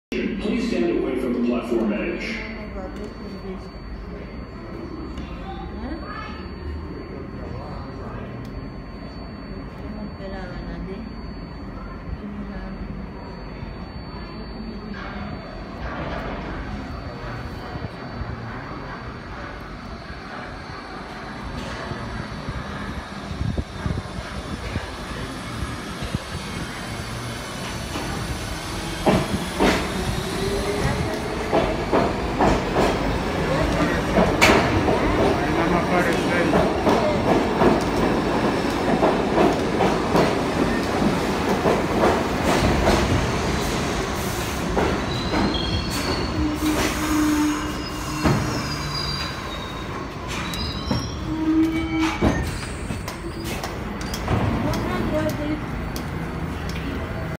New York City Subway | R62A 6 Local Train arriving at 125 Street
Monday, December 24th, 2018
Lexington Avenue & East 125th Street